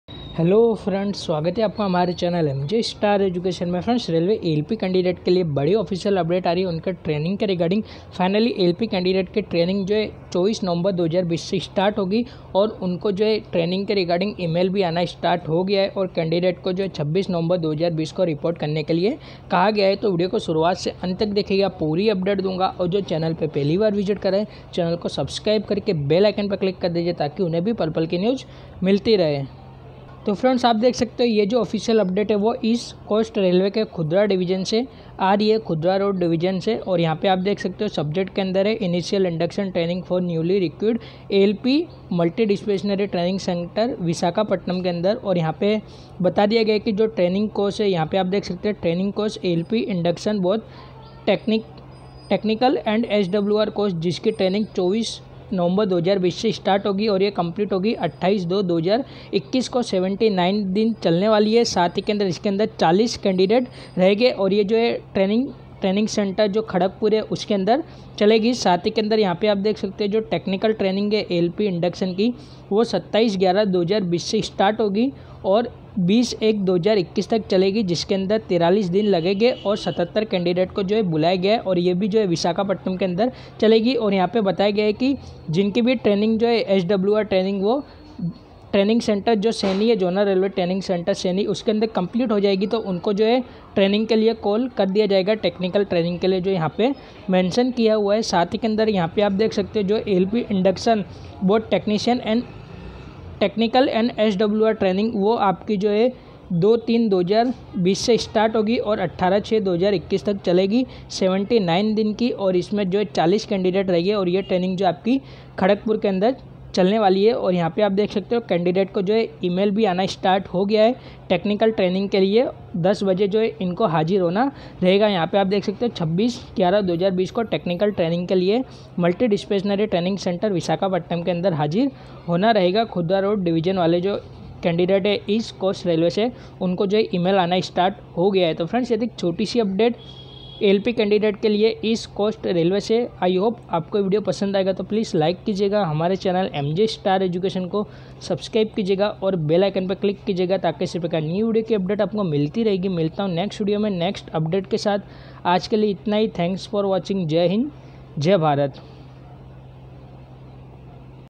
ALP CANDIDATE TRAINING START 24-11-20 से EMAIL आना स्टार्ट, रिपोर्टिंग 26-11-20 से ECR!
टेलीग्राम लिंक:- MJ STAR EDUCATION